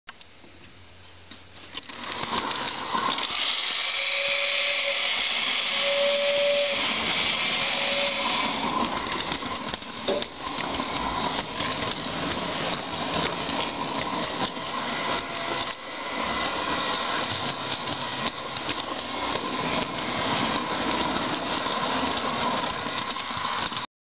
short train ride
camera rides on a flat bed